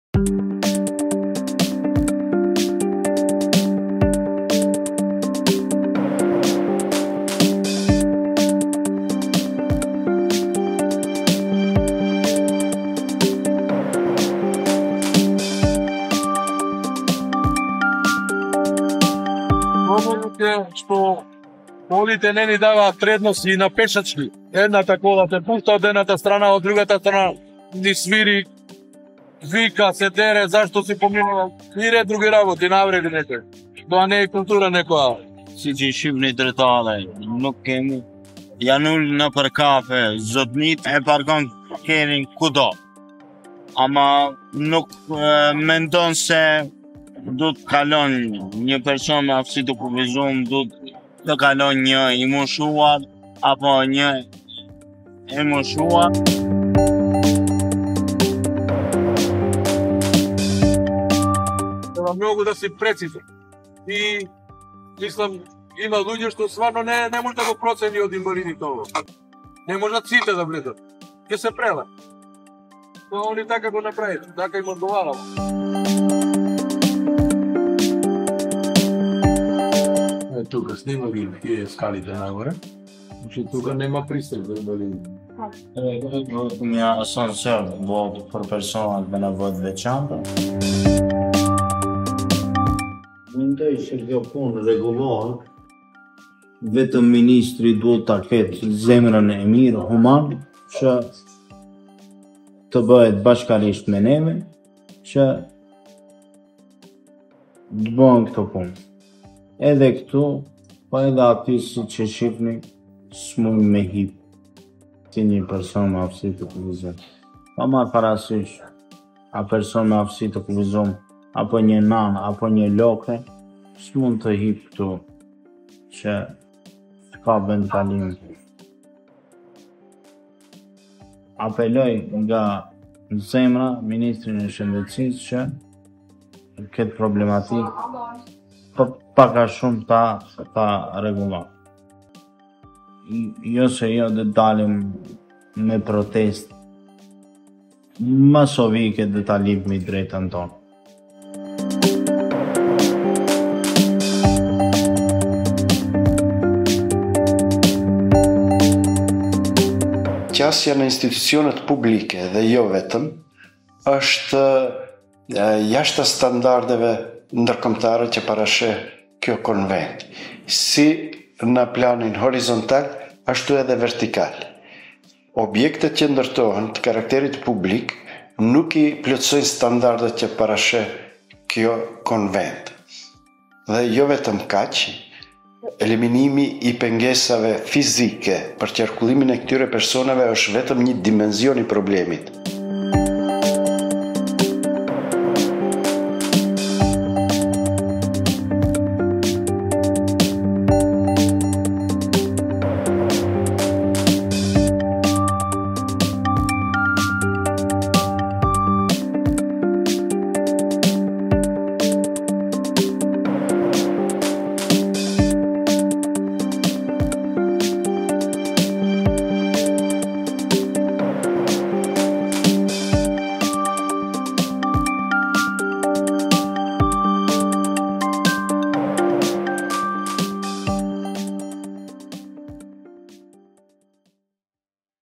Infrastruktura dhe institucionet në Çair mbeten armike për personat me aftësi të veçanta fizike
Një pjesë e institucioneve publike në rajonin e Çairit janë të paarritshme për personat me aftësi të veçanta, sidomos për ata në karroca. Rrugët dhe infrastruktura rrugore gjithashtu lë shumë për të dëshiruar. Disa nga sfidat e përditshme që këta qytetarë përjetojnë janë trotuaret e bllokuara nga veturat, mungesa e stazave të posaçme, mungesa e ashensorëve dhe të tjera. Ertan Shakiri dhe Zhivko Milevski nga shoqata “SOS Vita” për Portalb.mk treguan vështirësitë me të cilat ata përballen për çdo ditë. Përderisa ka ankesa te Komision për Parandalimi dhe Mbrojtje nga Diskriminimi vrejtje të mëdha ka edhe Avokati i Popullit. Por, ky grup i qytetarëve, vazhdon të mos jetë prioritet për institucionet. Lexo